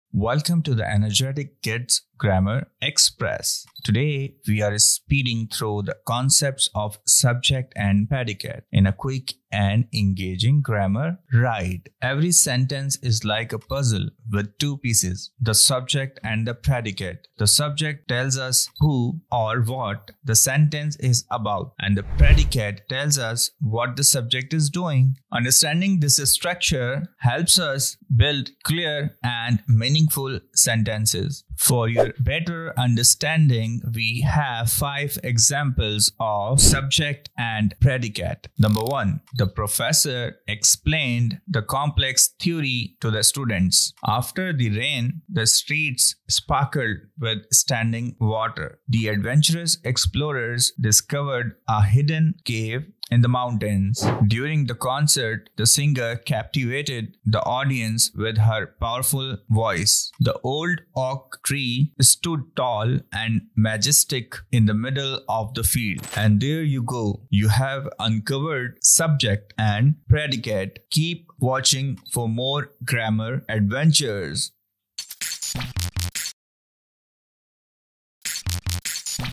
Mastering Language Brilliance: Unveiling the Power of Subject and Predicate
📚✨ Join us as we unravel the fundamentals of sentence structure and enhance your language skills.

🧐 Ever wondered about the backbone of every sentence? Look no further! Learn the importance of subjects and predicates in constructing clear and effective sentences.

🚀 In this episode, we break down complex grammar concepts into easy-to-understand examples, making it perfect for learners of all levels. Whether you're a student aiming for academic success or a language enthusiast, this video is tailored for you!

🌐 Enhance your linguistic journey and share this insightful video with your friends!

🌟 Let's spread the knowledge and love for language across Asia and beyond! 🌏💬